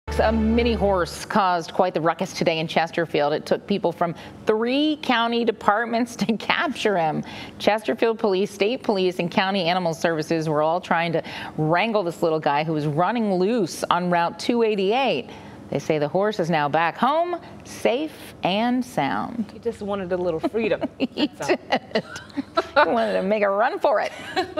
Miniature horse caught running loose on Route 288
Officials caught a miniature horse that was running loose around Route 288 Wednesday.